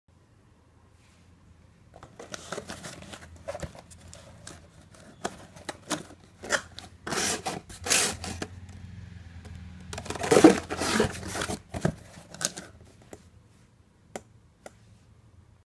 9X6X4 INCH POSTAL BOXES
Single wall Boxes made of twin flute layer for a better protection of good. Strong and fragile items can be very well secured in these boxes.